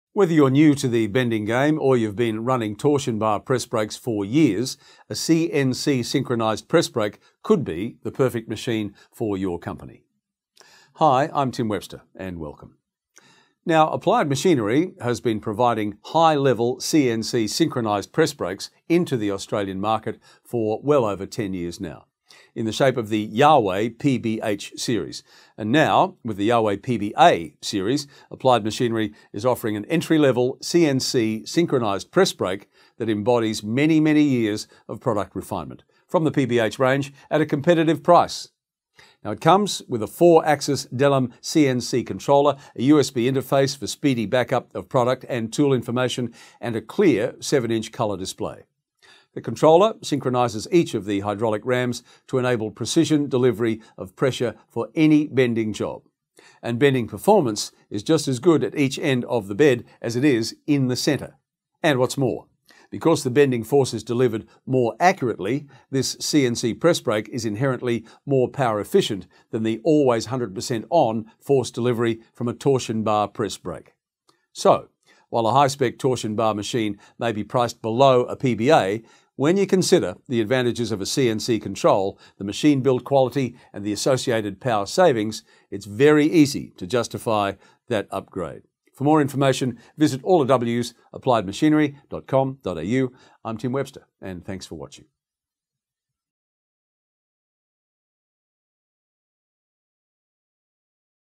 Industry Update: Applied Machinery - CNC Synchronised Press Brake
Tim Webster and Industry Update Manufacturing Media present the CNC Synchronised Press Brake, from the Yawei PBA Series, available in Australia from Applied Machinery.